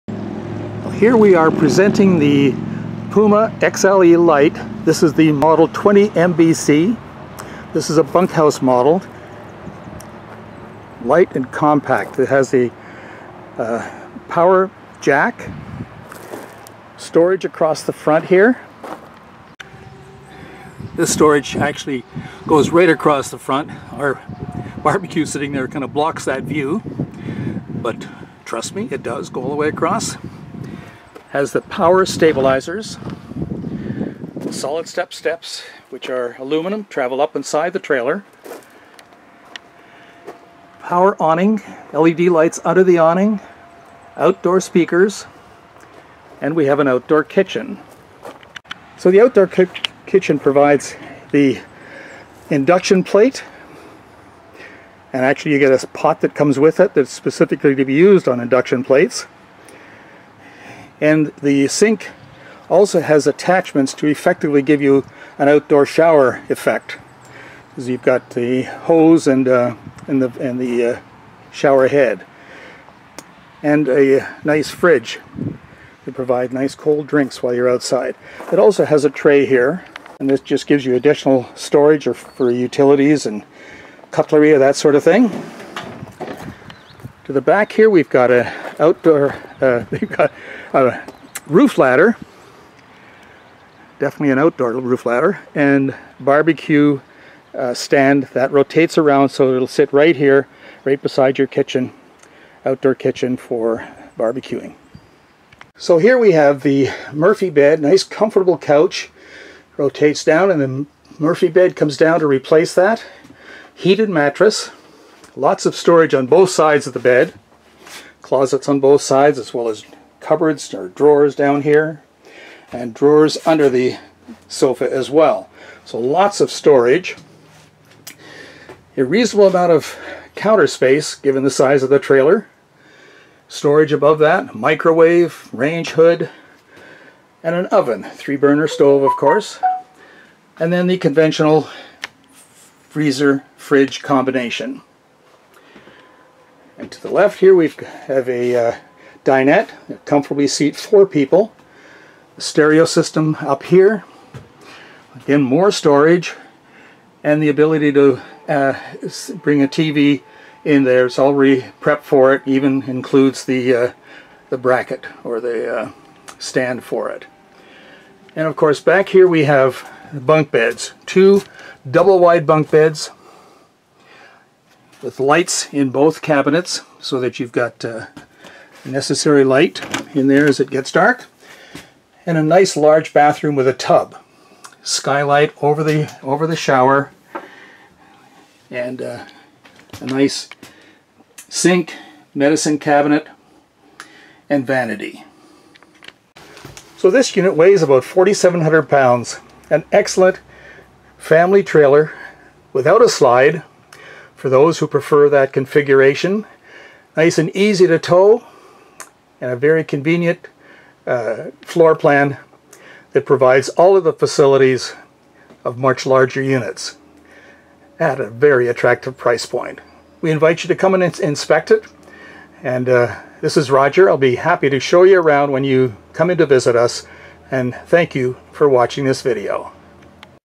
2019 PUMA XLE 20MBC
GREAT BUNK MODEL WITH A MURPHY BED AND LOTS OF ROOM IN A SMALLER TRAILER!!